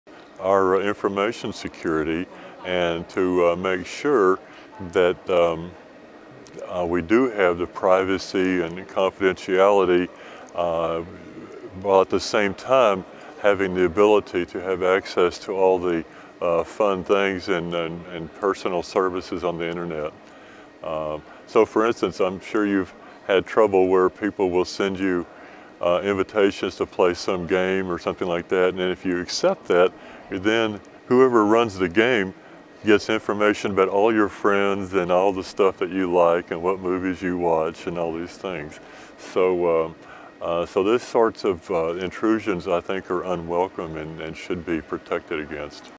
Best Action for Future? IETF 2015: Charlie Perkins
Charlie Perkins, Senior Principal Engineer of Futurewei, shares his answer to the question “What action can be taken now or soon to ensure the best possible future? Explain.” at Internet Engineering Task Force meeting 92, which took place in March 2015 in Dallas, Texas. Recorded by the Imagining the Internet Center, an initiative of Elon University, North Carolina, USA.